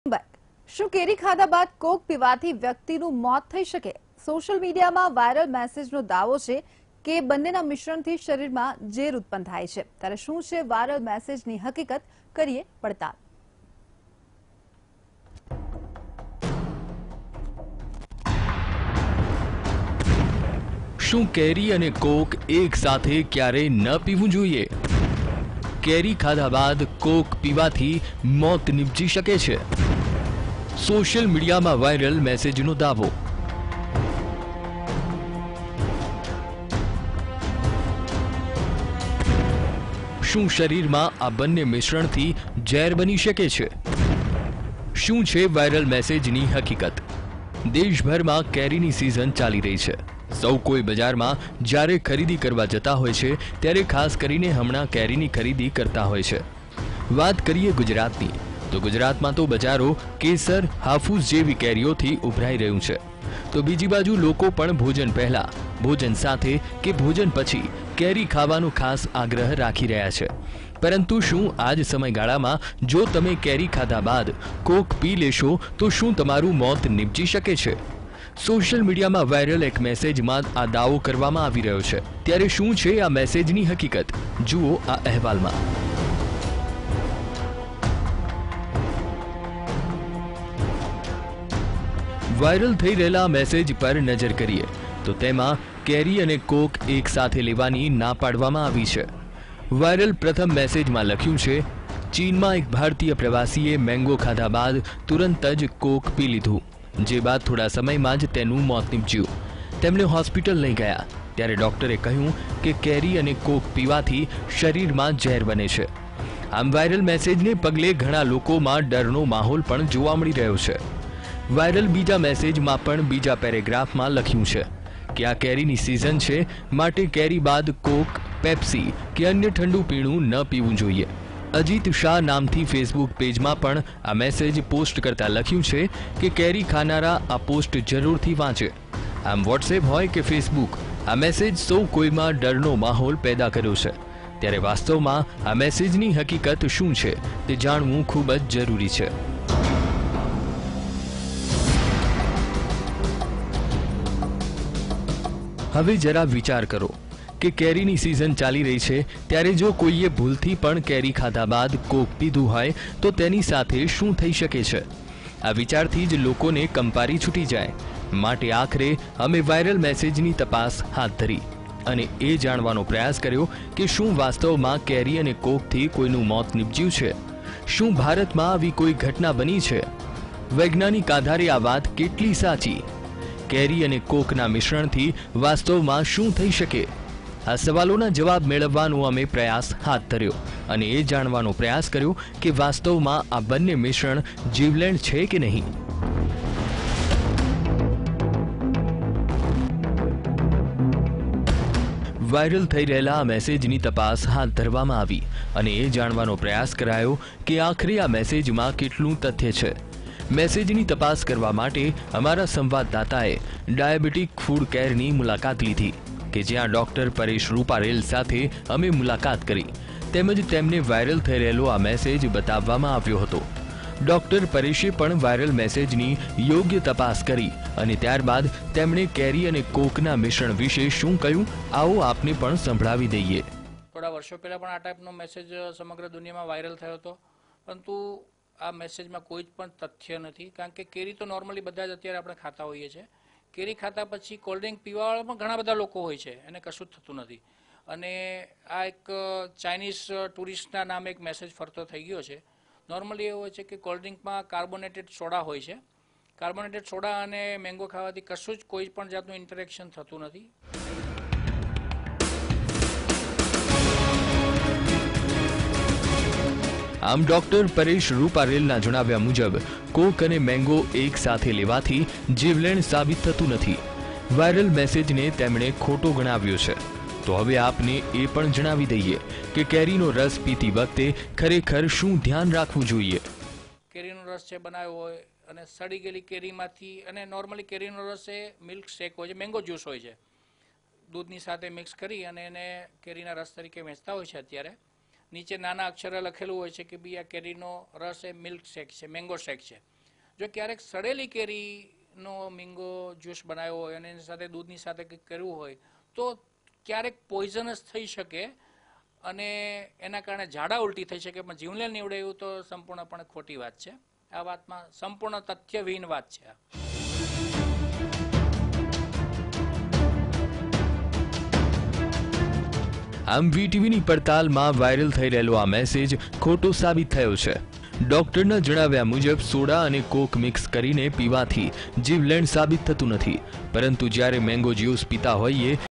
Viral Truth: Of You will be died if Drinking coca cola after eating mango fruit | Vtv News
Viral Truth: Of You will be died if Drinking coca cola after eating mango fruit.